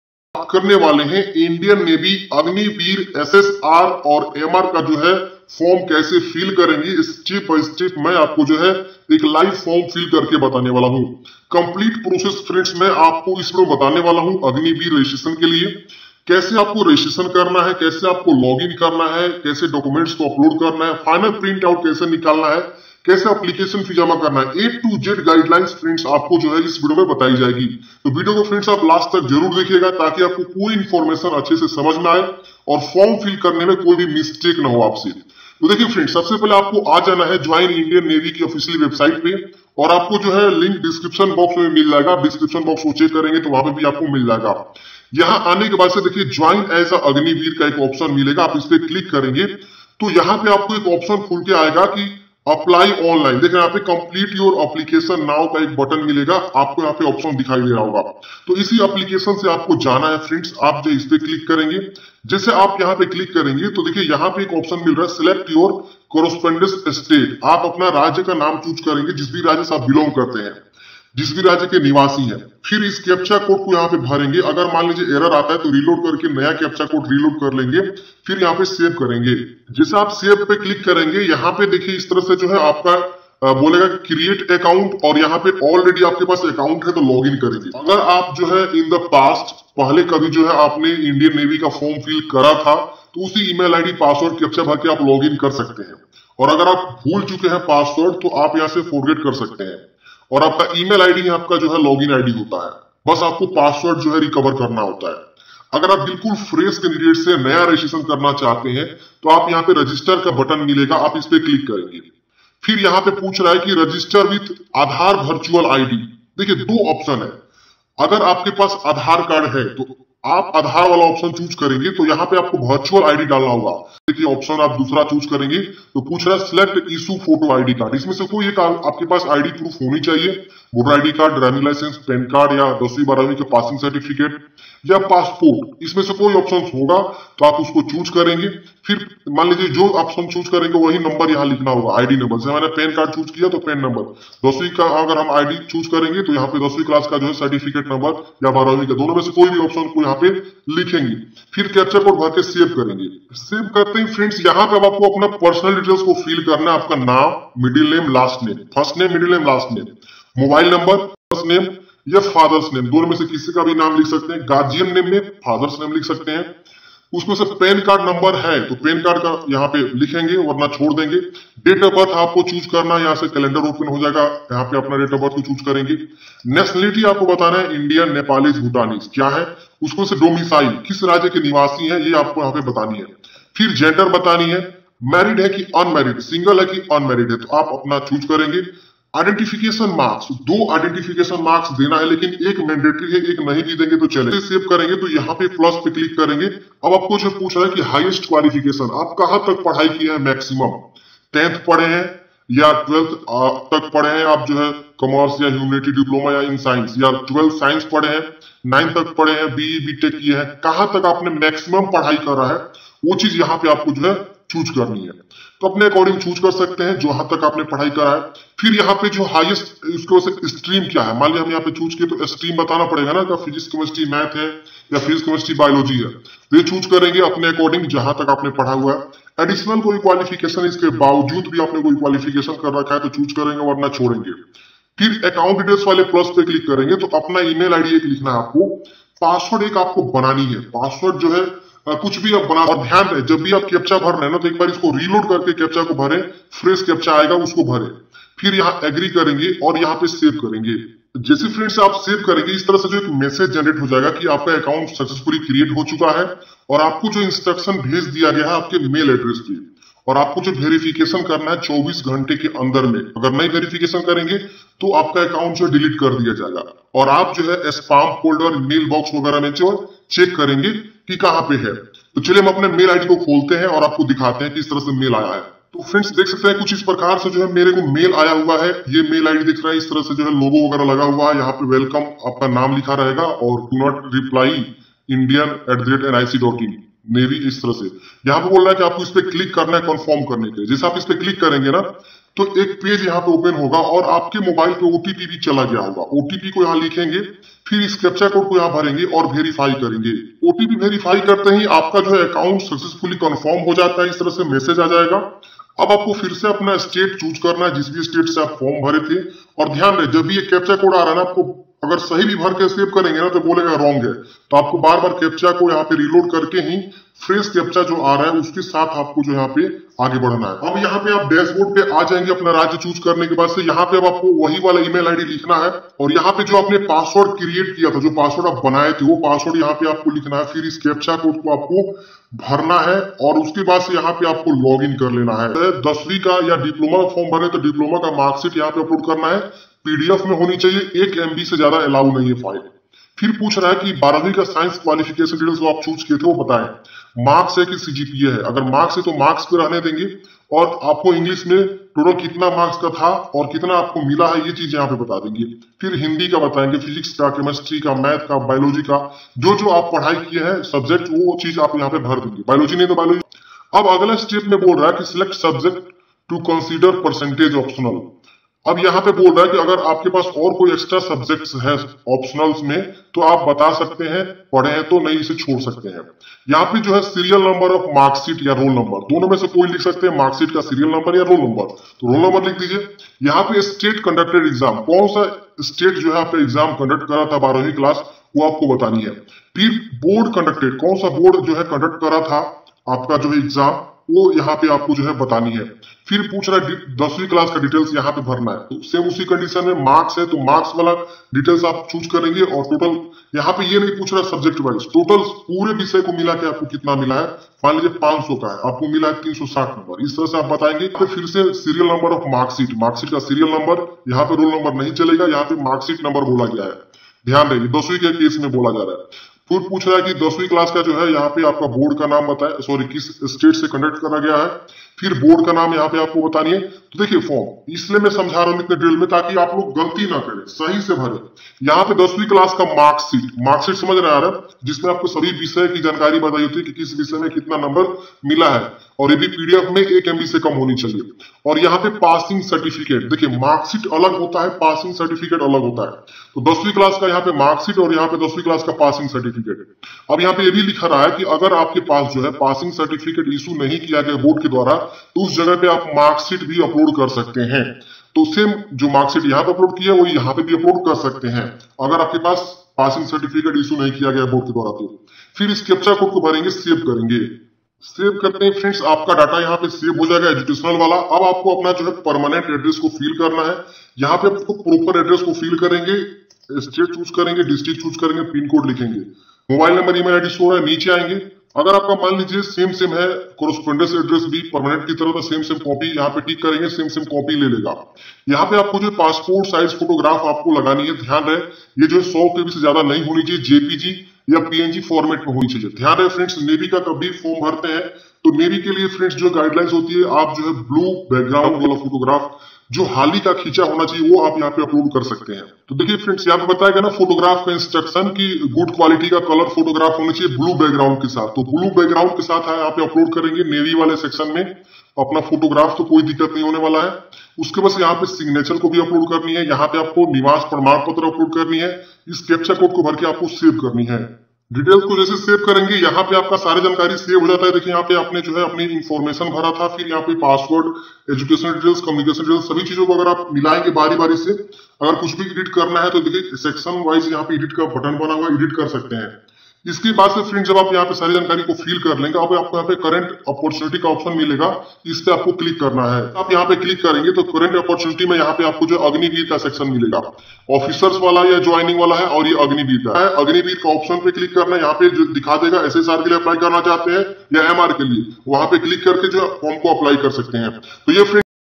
Indian Navy Agniveer SSR Online Form 2022 Kaise Bhare ¦¦ How to Fill Navy SSR Online Form 2022 Apply
वायुसेनाभर्ती 2022
# 10th,12thpassjob
Homeguardrecruitment 2022
RPF Recruitment 2022
no exam
forest guard
forester ranger
DRDO job
job with out exam
DRDO recruitment 2022